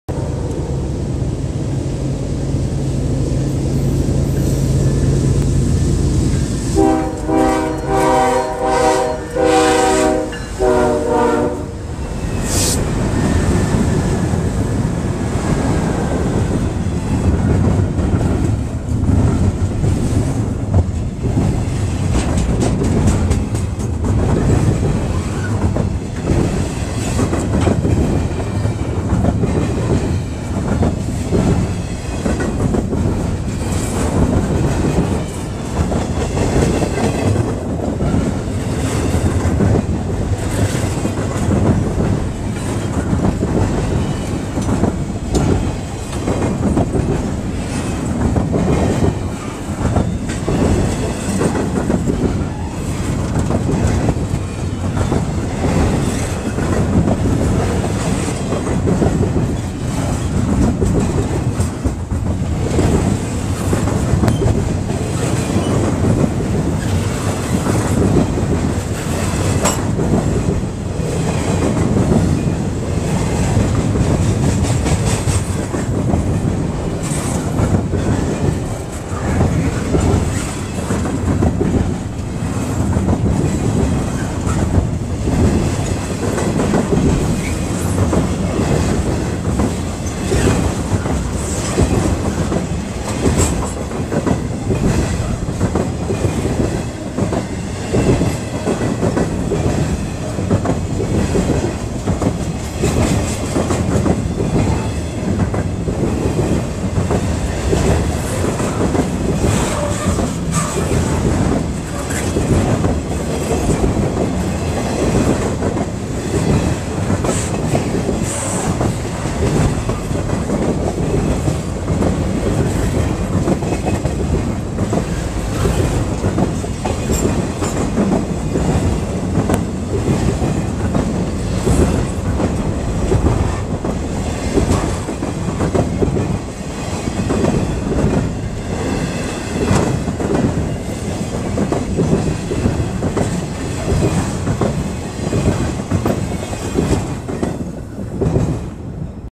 BNSF 7616 leads Intermodal at Joliet
BNSF 7616(ES44DC), BNSF 1110(C44-9W), BNSF 4790(C44-9W), and BNSF 4888(C44-9W) lead an intermodal south on the Chilicothe Sub. in Joliet